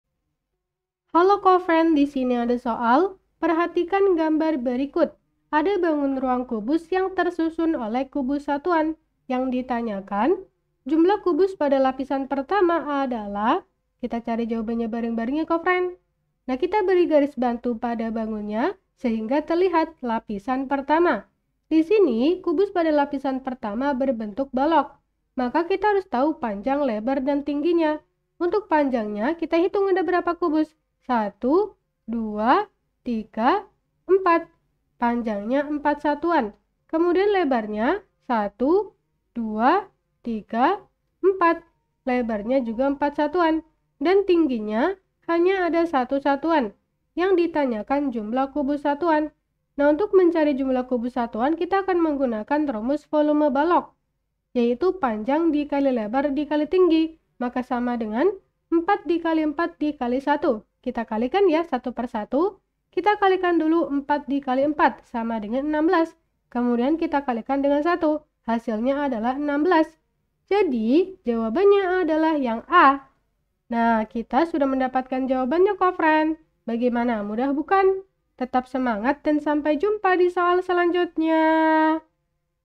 Perhatikan gambar berikut. Jumlah kubus pada lapísan pertama adalah .... a. 16 C. 8 b. 12 d. 4
Perhatikan gambar berikut.
Jumlah kubus pada lapísan pertama adalah .
a. 16
C. 8
b. 12
d. 4

Topik atau Materi: Volume dengan Kubus Satuan - Bangun Ruang - Geometri - Matematika Kelas 5 SD - K-13 Revisi

Sumber Soal: MBP Matematika Kelas 5, K-13 Revisi, TMBP, 2021 , Masmedia Buana Pustaka

00:00 : Bedah Soal
01:00 : Konsep, rumus dan pengertian Volume dengan Kubus Satuan
01:18 : Langkah penyelesaian soal
00:22 : Jawaban akhir
00:52 : Penutup